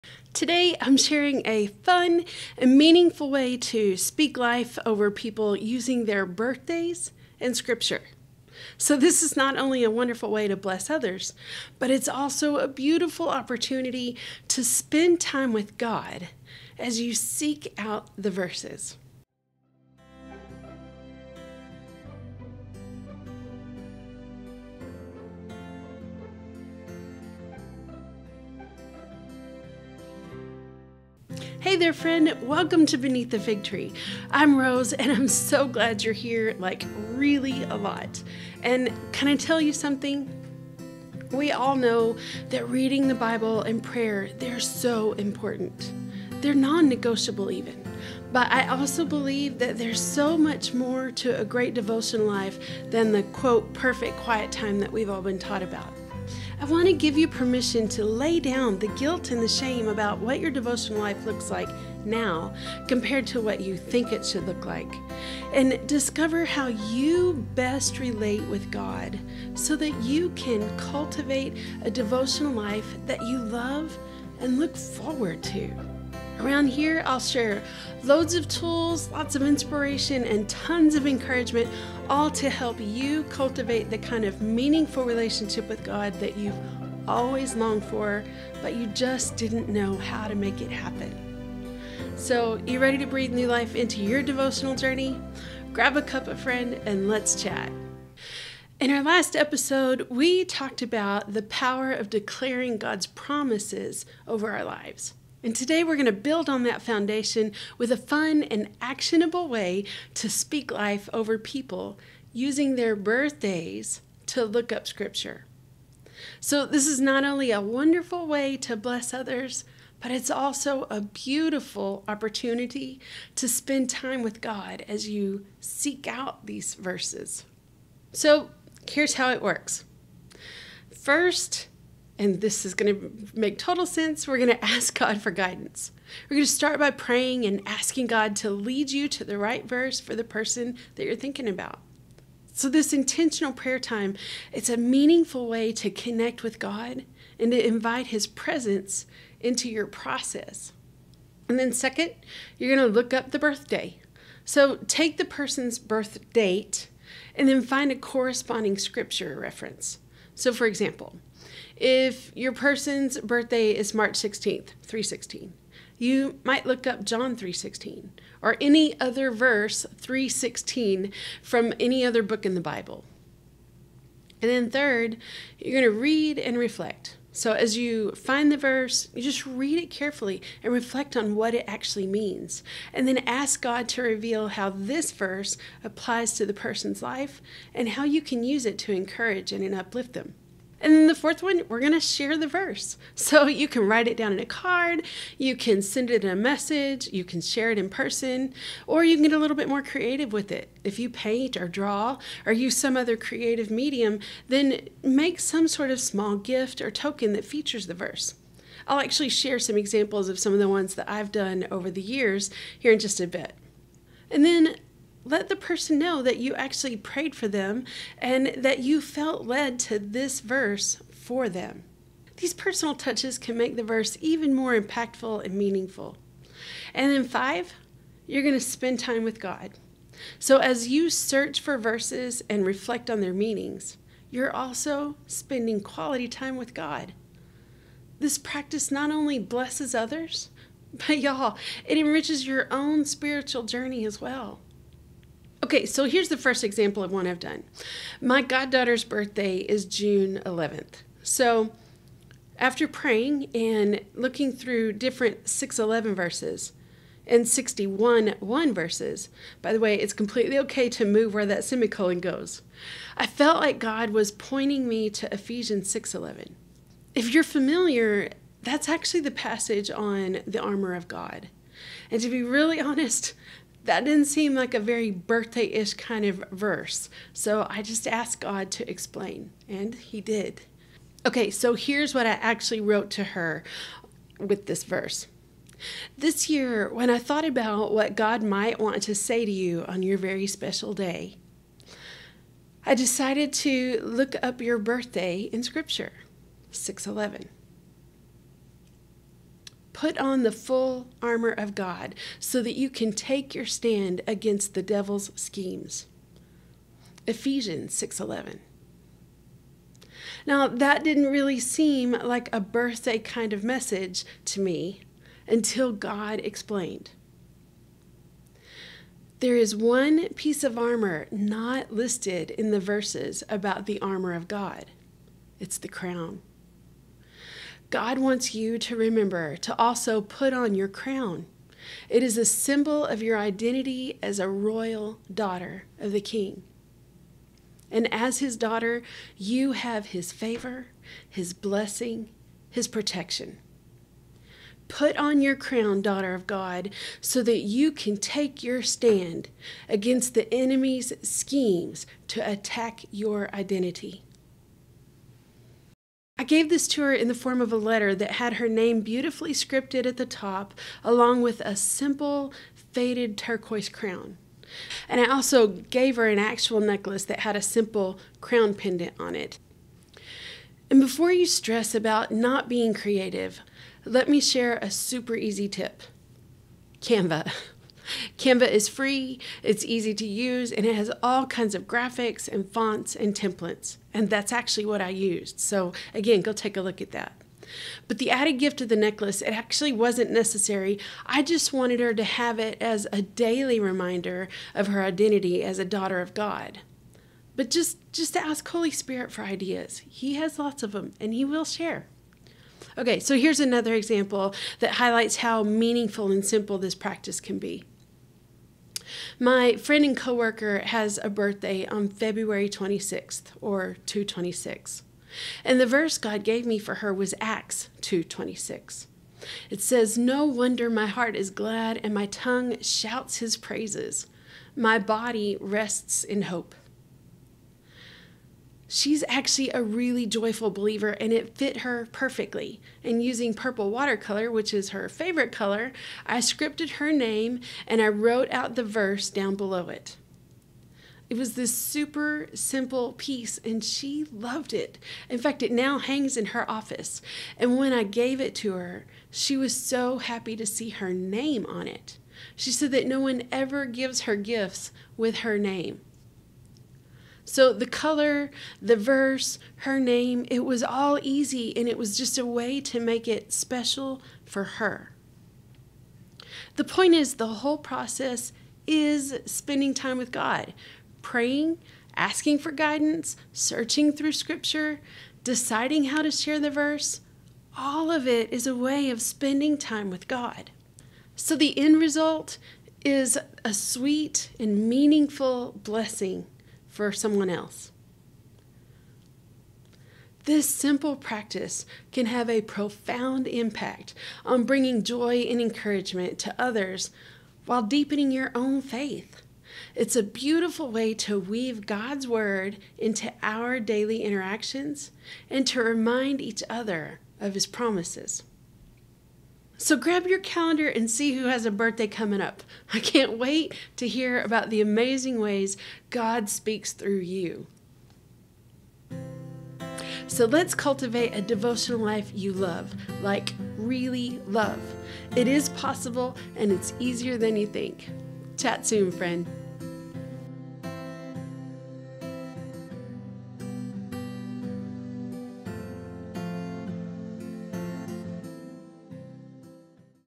Birthday Blessings & Bible Truths
Today, I’m sharing a fun and meaningful way to speak life over people using their birthdays and scripture. This is not only a wonderful way to bless others, but it’s also a beautiful opportunity to spend time with God as you seek out these verses.

LINKS
✝️ TRANSCRIPT & SHOW NOTES